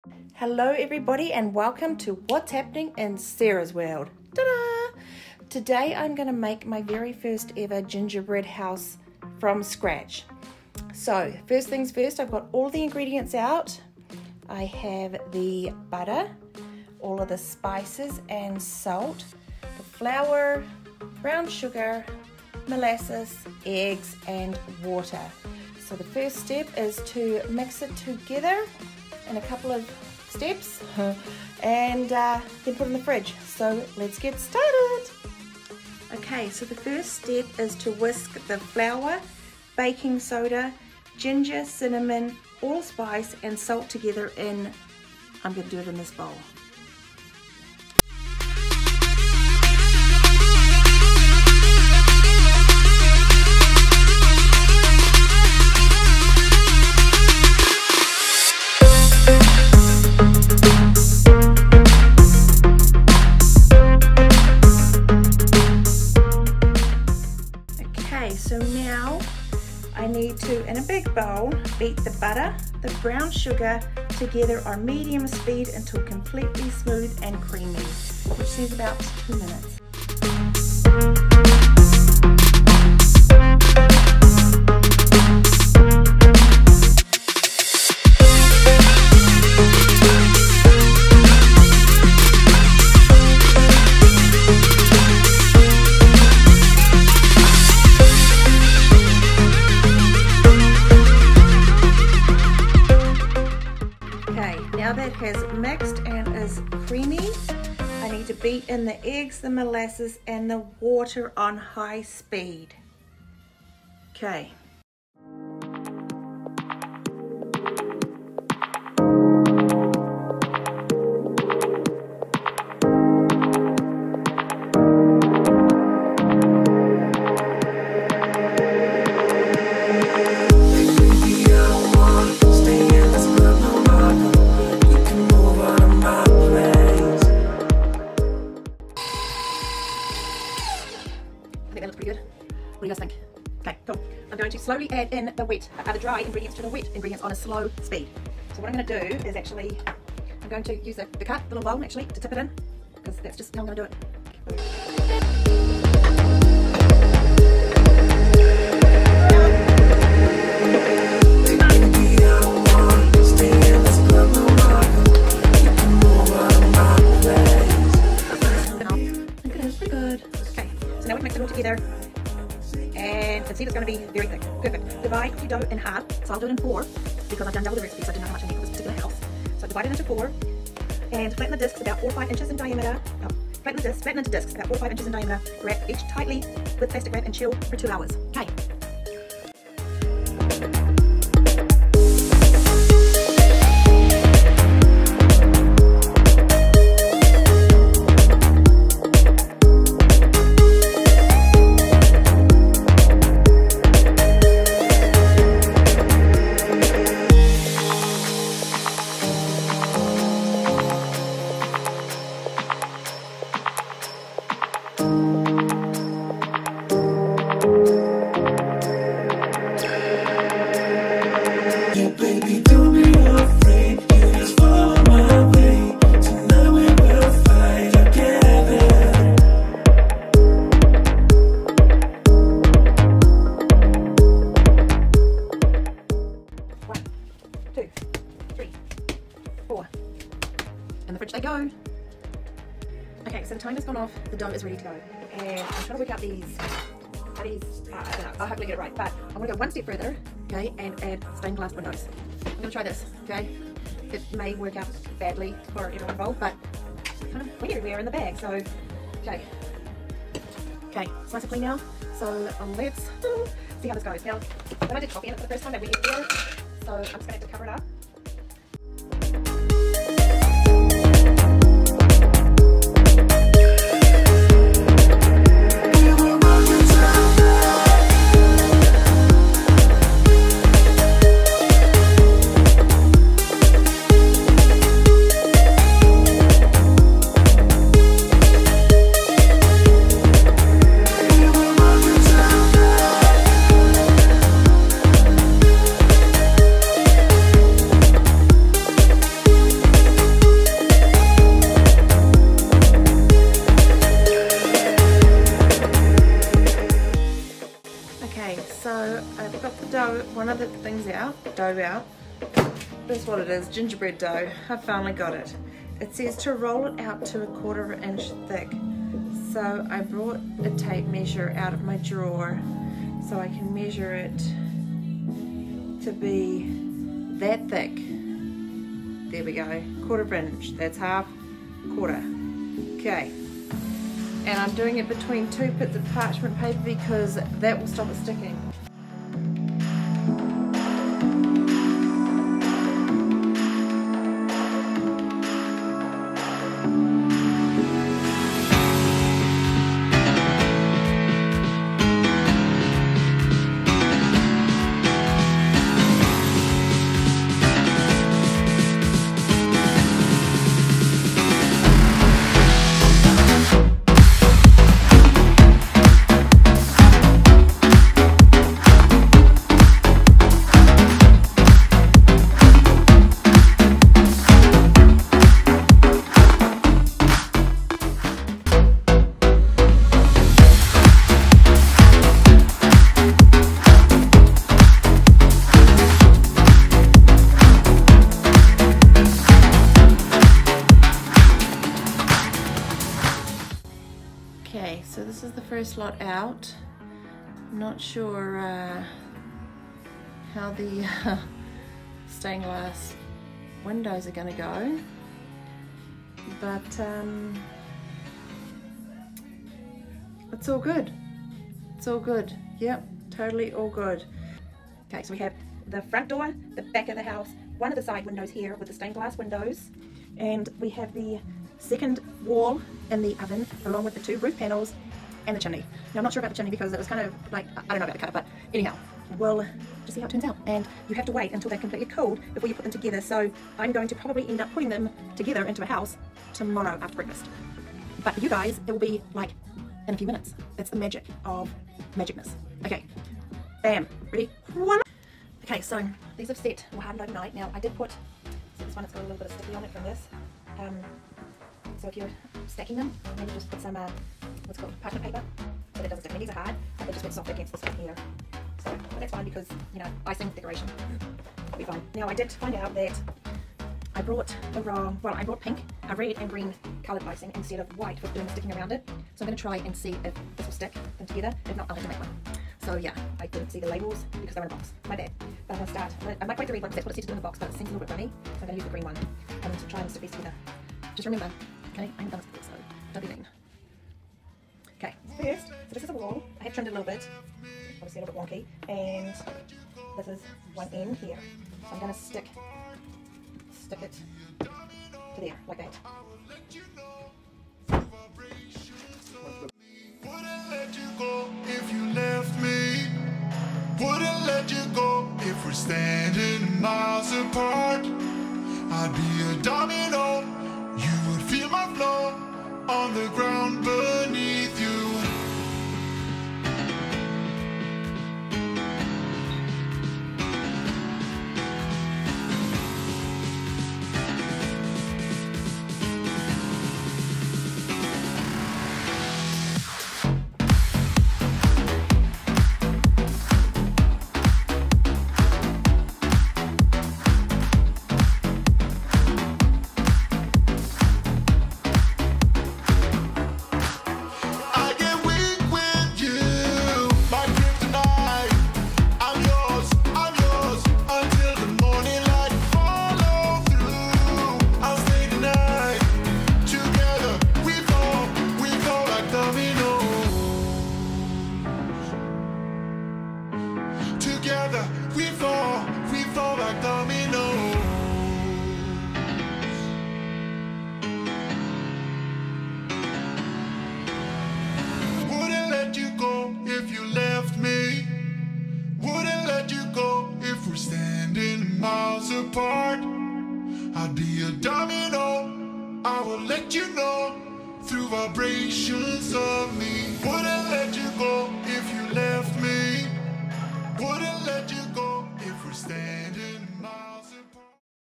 1st Gingerbread House Attempt!!
😊 Ok so while I am no Master Baker I do Love to Cook. Whether it's Treats or Dinner, I think the best thing I like about cooking is seeing others enjoy it. 😊

Even if it is just a decoration like a Gingerbread House it always has to taste GOOD.

Be Great!!

Ps: I am from New Zealand but now live in Utah so occasionally I bake New Zealand Treats ❤️ 🇳🇿 ❤️